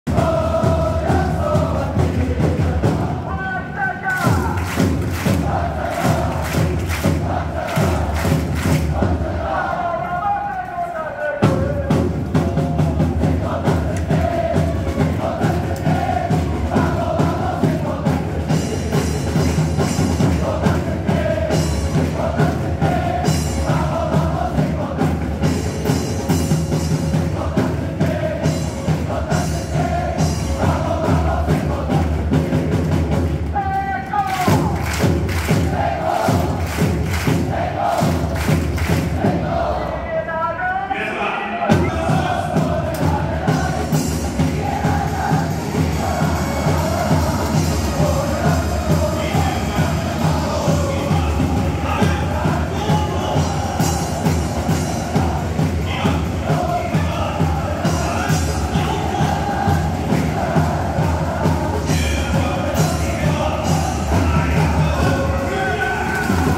試合前 選手チャント①
脇坂選手、瀬古選手、家長選手
【川崎フロンターレ】天皇杯新潟戦